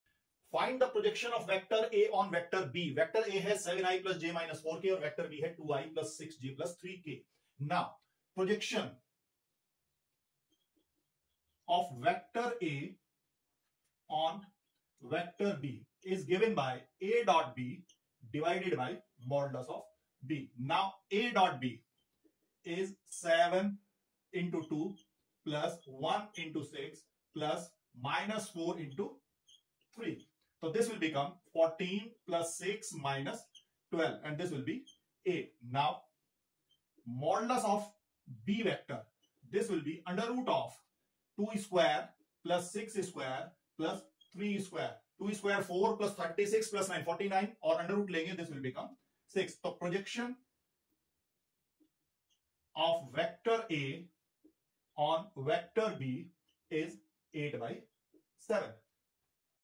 Projection of a Vector | Scalar Product | Dot Product | CBSE Board Exam 2024 | CUET 2024
In this short video, we'll learn how to find the projection of one vector onto another. Specifically, we'll work through an example problem where we find the projection of the vector a =7i ̂+j ̂-4k ̂onto the vector b=2i ̂+6j ̂+3k ̂.

First, we'll review the concept of vector projection and explain how it can be used to find the component of one vector that lies in the direction of another vector. Then, we'll walk through the steps to find the projection of a onto b using the dot product and vector algebra.

This video is intended for anyone who wants to learn more about vector mathematics, including students studying calculus, physics, or engineering. By the end of the video, you'll have a solid understanding of how to find the projection of one vector onto another, and you'll be able to apply this concept to solve similar problems on your own.

To learn Mathematics always follow DeCode Maths with SAJAG JAIN